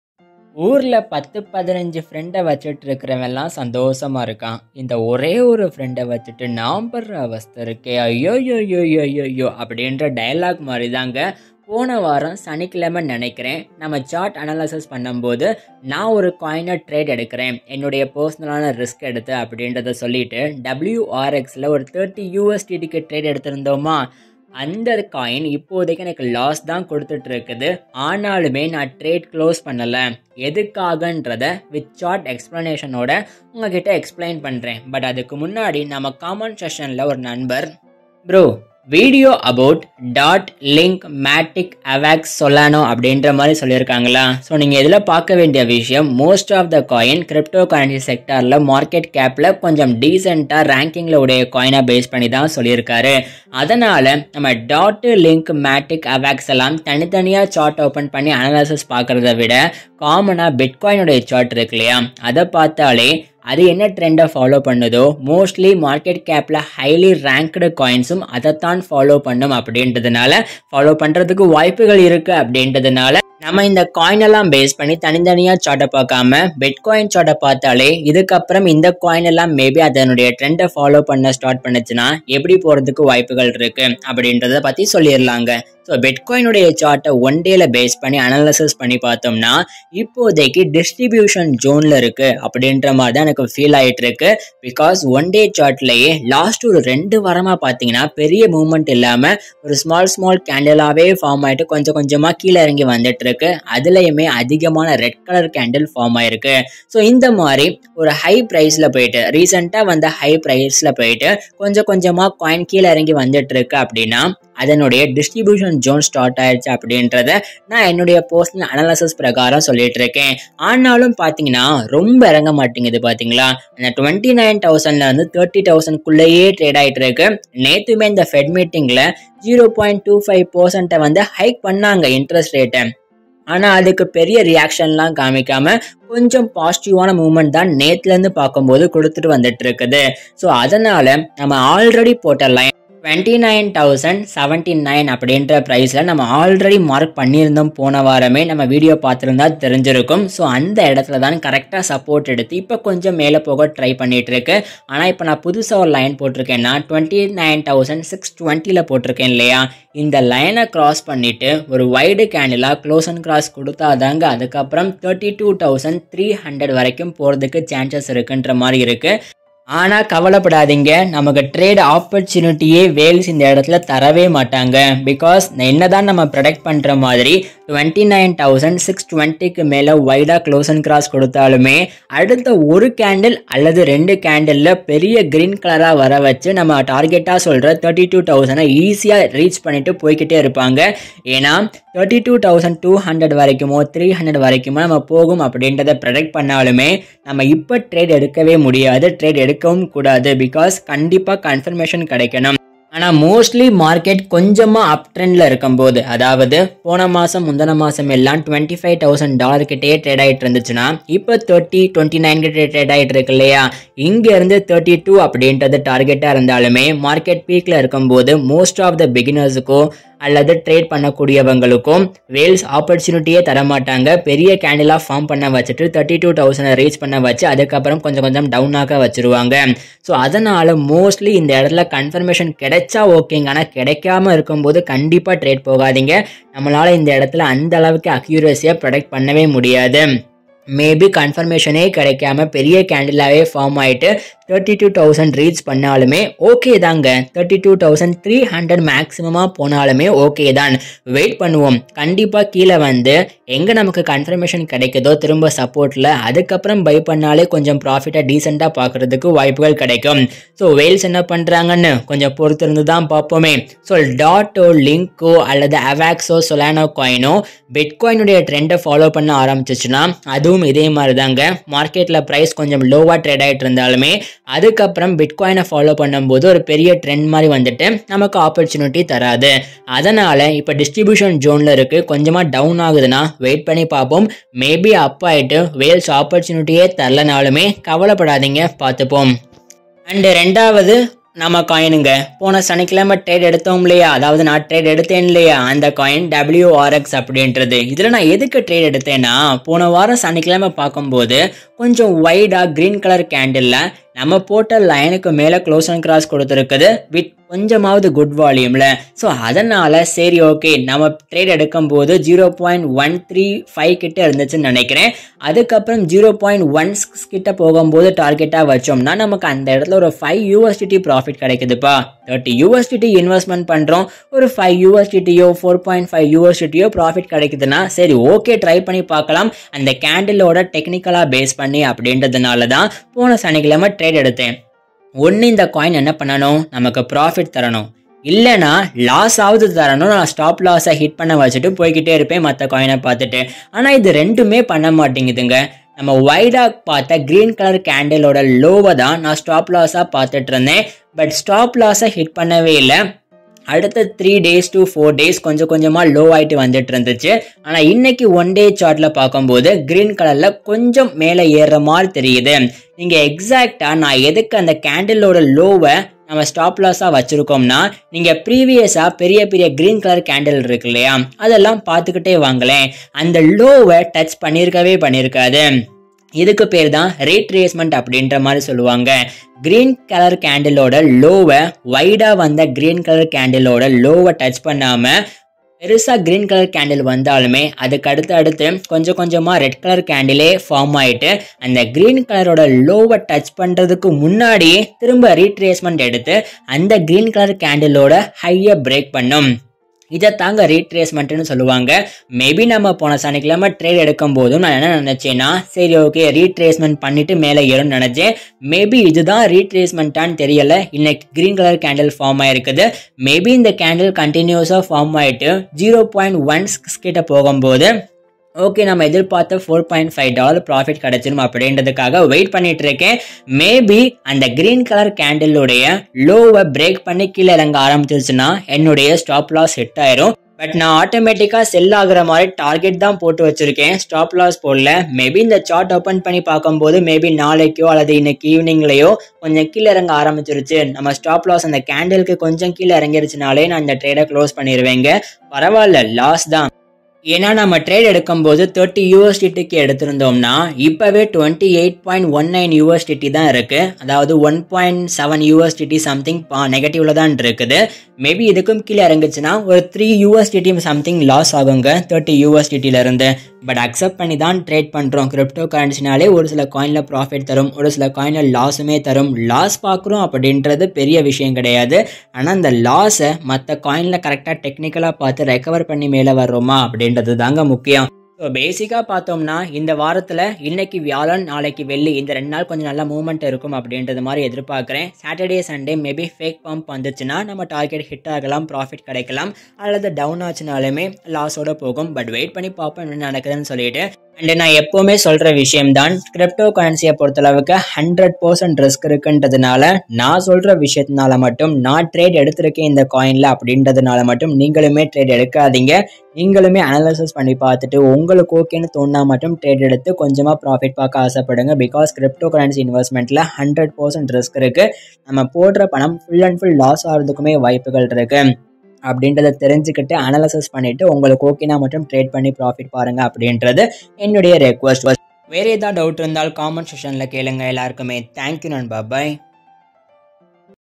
Wazrix & Bitcoin Crypto Price analysis 2023 (Tamil)
Wazrix & Bitcoin Crypto Price analysis 2023 (Tamil) | Mr.Coin

In this video I explained that I traded wrx coin and bitcoin price analysis in my points of view prediction elaborated then I'm not a Finacial advisor so this video is for educational purposes only.